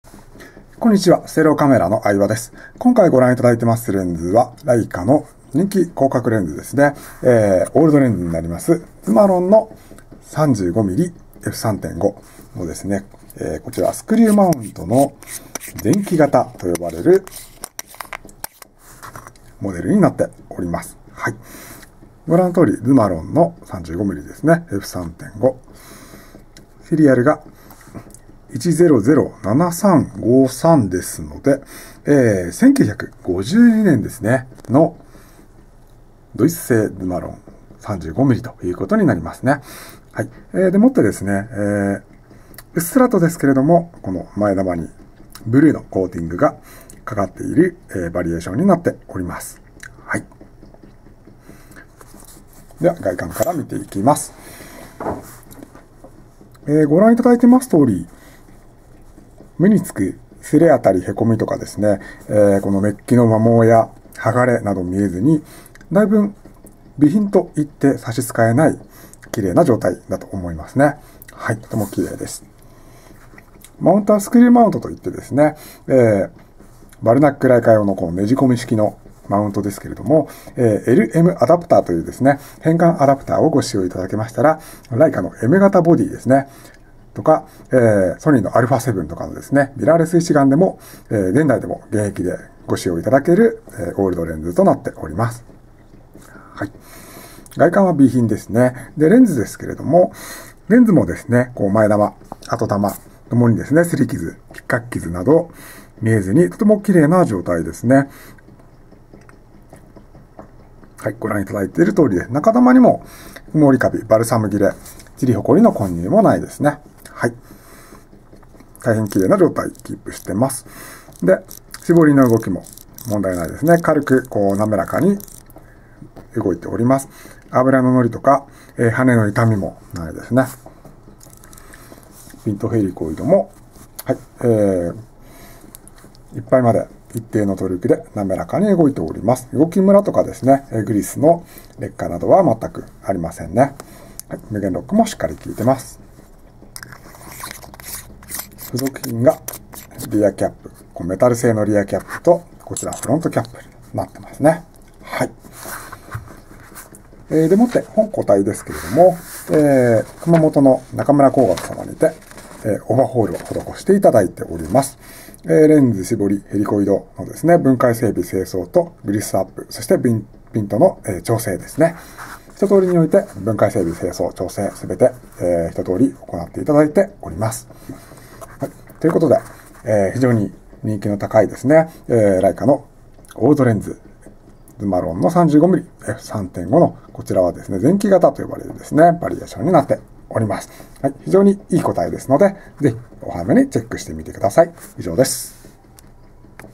★LEICA ライカ Summaron ブルーコーテッド ズマロン 35mmF3.5 L 前期
戦後ライツが最初に手がけたレンズ
中村光学OH済（整備票付属）、前後キャップ付